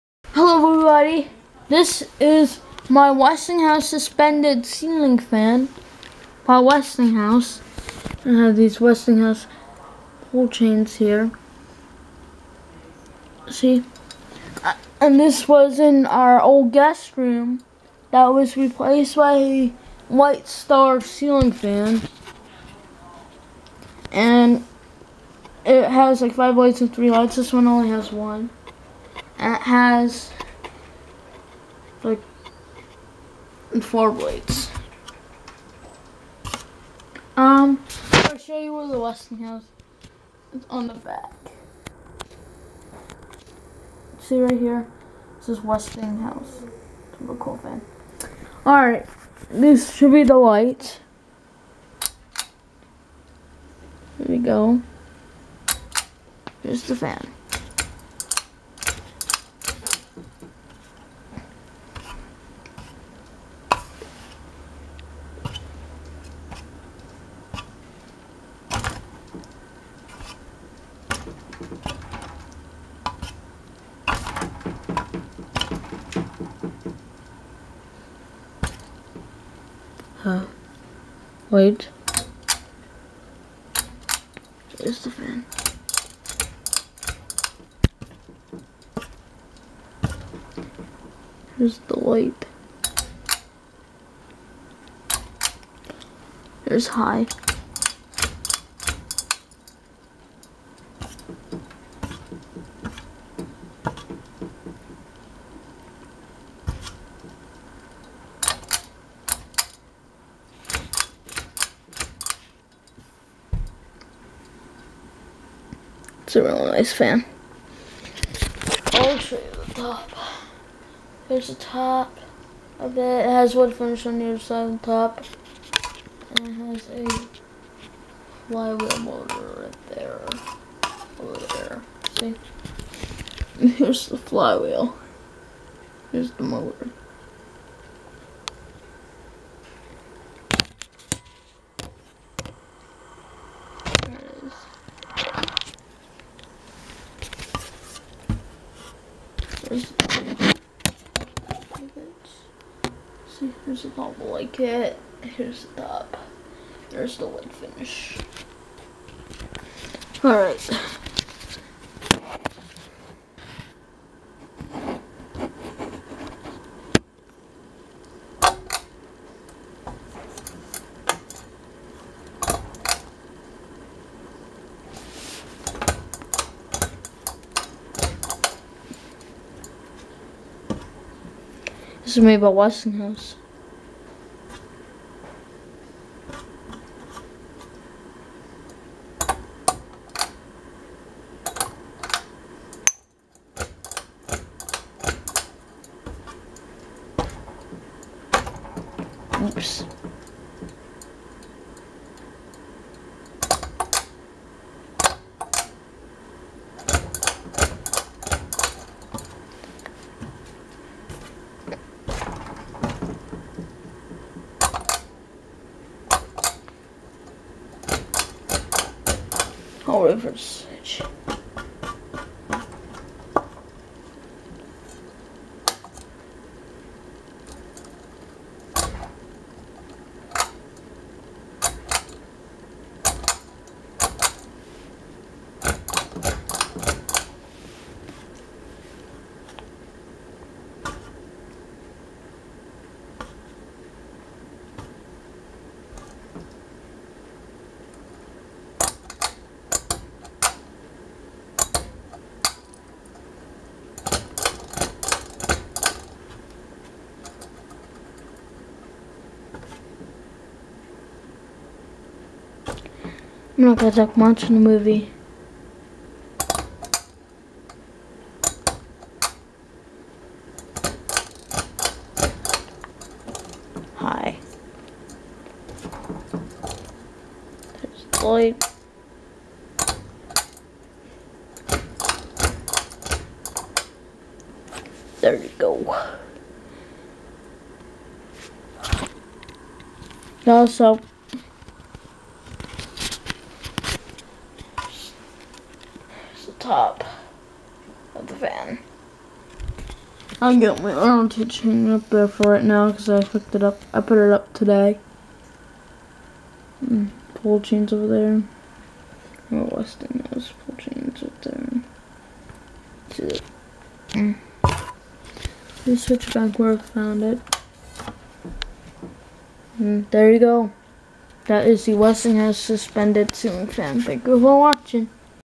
Westinghouse Nova ceiling fan (January 2016)
The Westinghouse Nova ceiling fan is not hooked up.